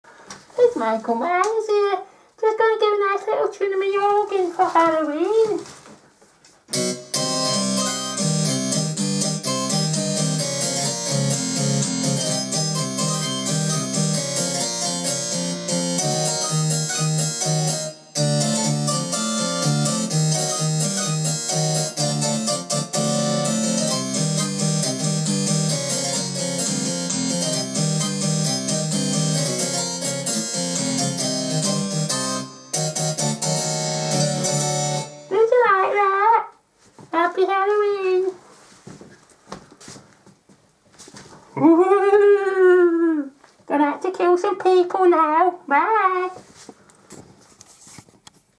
Michael Myres Plays a Spooky Tune For Halloween
HI guys..I thought I'd get you all in the Halloween mood with a little tune I made up on my organ